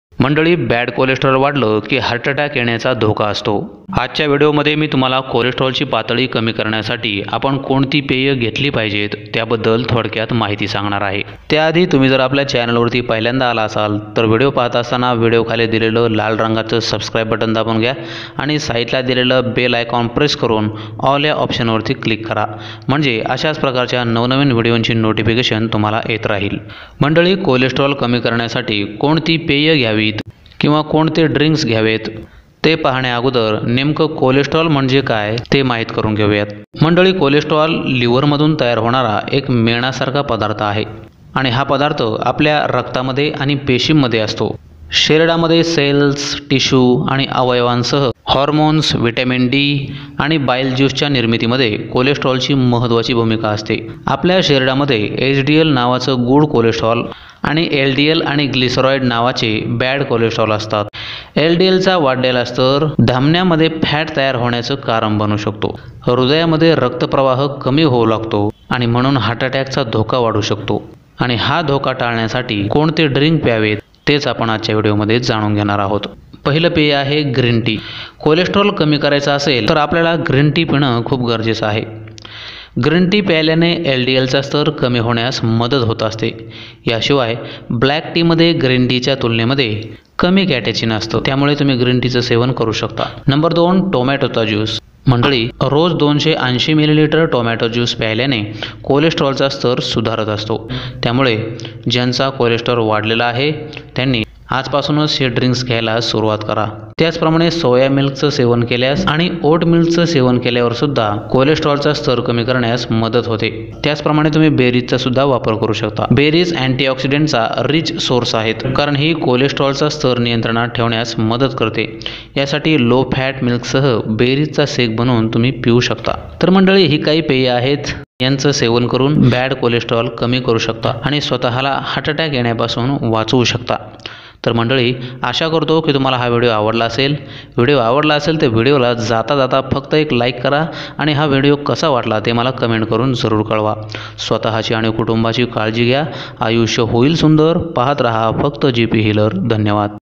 कोलेस्ट्रॉलची पातळी कमी करण्यासाठी फक्त ही प्येयं घ्या | Bad Cholesterol | Heart Attack❤💓
नमस्कार मंडळी आजच्या व्हिडिओमध्ये मी कोलेस्ट्रॉलची पातळी कमी करण्यासाठी कोणती प्येयं घ्यावित किंवा प्यावित ते सांगण्याचा प्रयत्न केला आहे.
माहिती साठी आहेत.कोणताही उपाय करण्याआधी तज्ञांचा सल्ला जरुर घ्या.